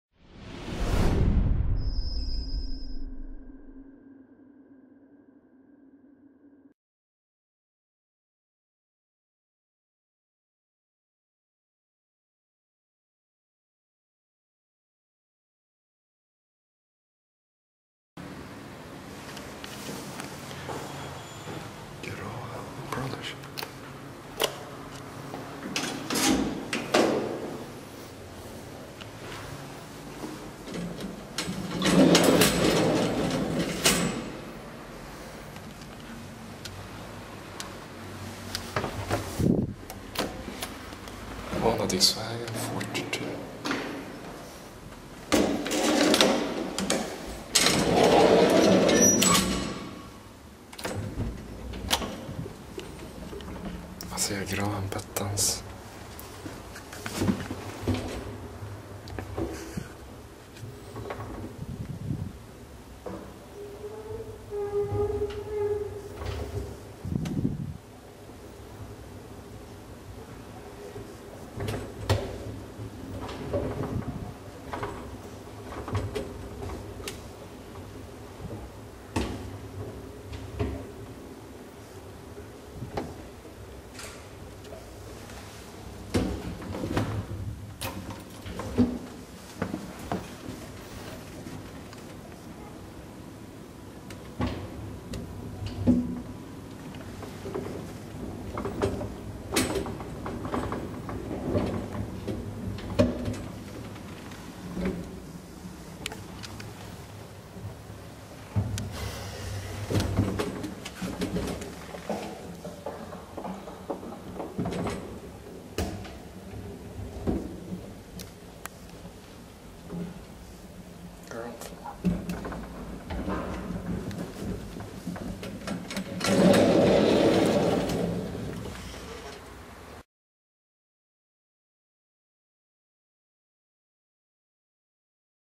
1912 Grahams brothers elevator @ Vanadisvägen 42
New set of fixtures has been made during the 60´s.